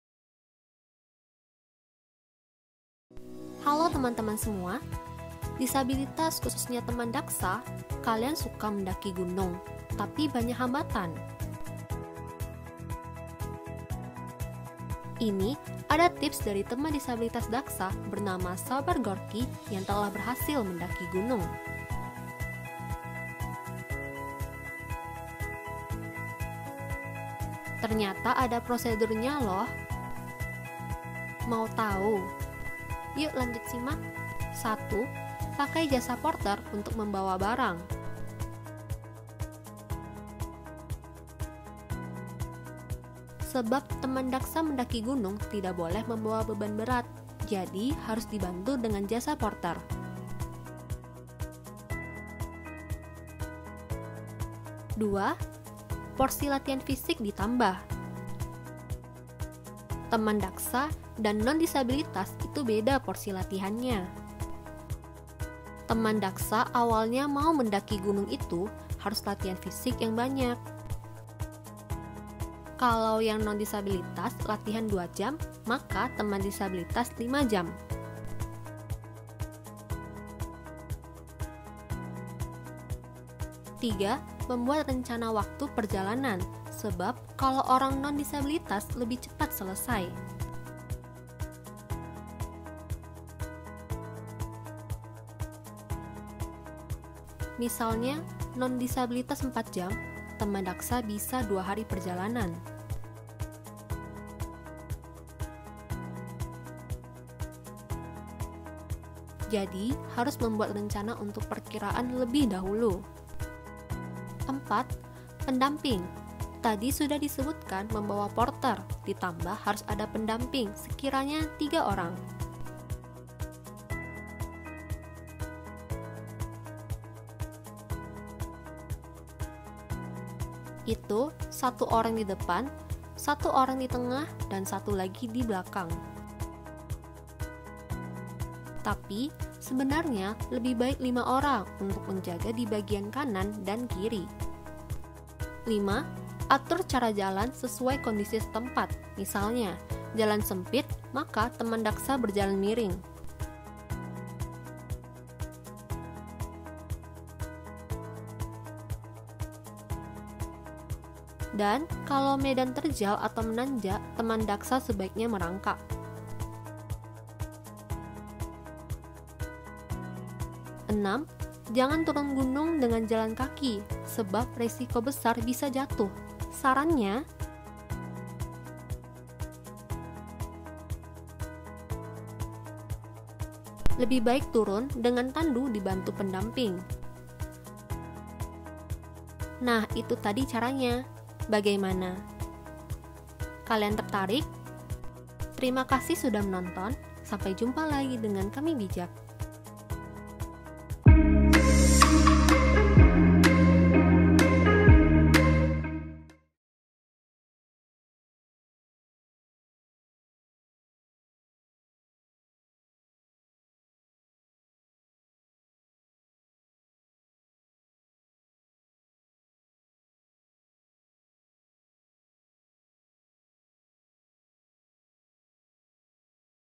Teman Disabilitas Ingin Mendaki Gunung? Yuk Ikuti Tips Berikut!
Kamibijak.com, Hiburan – Aktivitas mendaki gunung adalah kegiatan yang cukup ekstrim. Medan yang curam, dan tekanan udara yang tidak sama adalah dua dari banyak tantangan yang harus dihadapi sebelum mencapai ke puncak.

Tidak hanya orang-orang pada umum yang bisa mendaki gunung, teman Disabilitas juga bisa. Seperti yang dilakukan Sabar Gorky, seorang teman Daksa.

Sabar Gorky adalah teman Disabilitas daksa yang mampu mendaki gunung. Ia membuktikan bahwa memiliki kekurangan bukan penghalang segalanya.

Irfan Ramdhani, pria yang berusia 30 tahun adalah salah satu orang yang berhasil terinspirasi dari Sabar Gorky. Irfan mengalami disfungsi kaki kiri karena terjatuh dari ketinggian setinggi 10 meter saat mengikuti latihan single rope technique pada tahun 2010 lalu. Ia seingkali mengikuti kegiatan pencinta alam.

"Ternyata ada prosedurnya buat pendaki Disabilitas Daksa yang mau naik gunung," kata Irfan Ramdhani kepada Tempo pada 26 Juni 2020.

Latihan fisik teman Disabilitas harus lebih sering dilakukan ketimbang teman non-disabilitas. Khusus teman-teman daksa, mereka harus menambah latihan fisik.
"Jika non-disabilitas dua jam, saya lima jam," jelas Irfan.

Merancang waktu perjalanan juga harus dilakukan bagi teman Disabilitas, dikarenakan pendaki Disabilitas bisa memakan waktu lebih lama. Saya dari Ranu Pane ke Ranu Kumbolo sampai dua hari, yang non-disabilitas sekitar empat jam," papar Irfan.

Teman-teman Disabilitas harus didampingi setidaknya tiga orang dengan strategi satu di depan, satu di tengah, dan satu di belakang. Namun, teman Disabilitas bisa juga didampingi dengan sebanyaknya lima orang, dengan strategi dua orang di sisi kanan dan di sisi kiri pendamping teman Disabilitas.

Menurut penuturan Irfan, hal tersebut sangat berguna khususnya bagi teman Disabilitas daksa. Irfan beberapa kali sempat terjatuh di Gunung Semeru, kemudian Irfan diselamatkan oleh pendampingnya.

Penyesuaian melangkah harus disesuaikan karena teman Disabilitas berjalan dengan situasi medan yang cukup berbahaya. Menurut penjelasan Irfan, kecelakaan sering terjadi saat menuruni gunung. Irfan merekomendasikan untuk menggunakan jasa pemandu untuk membantu turun dari pendakian.